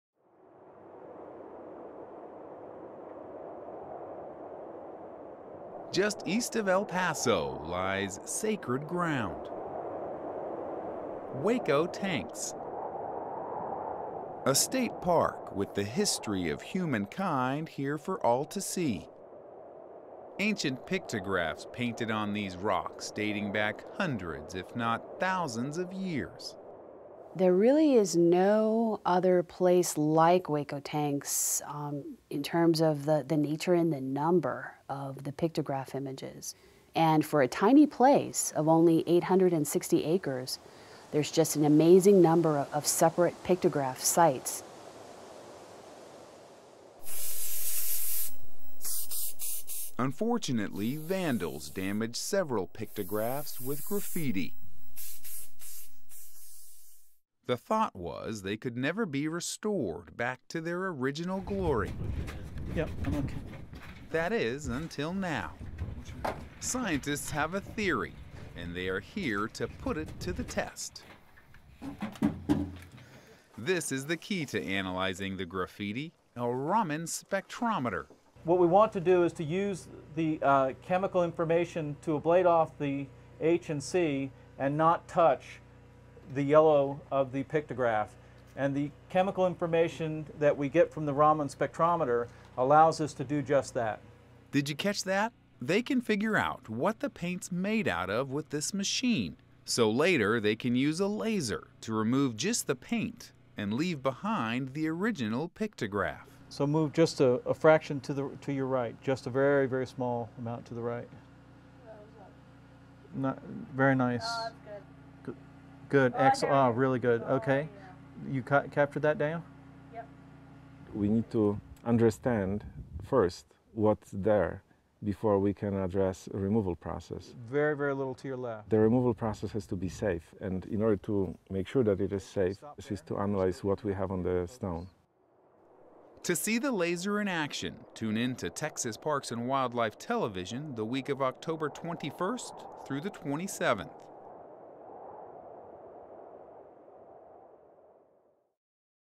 Hueco Tanks Rock Art and Laser Technology
New laser technology is being put to the test at Hueco Tanks State Park and Historic Site. Can lasers be used to remove graffiti paint and leave ancient pictographs unharmed? Here is a glimpse into this work that has never been tried before. The entire story will air on Texas Parks and Wildlife Television the week of October 21-27, 2012.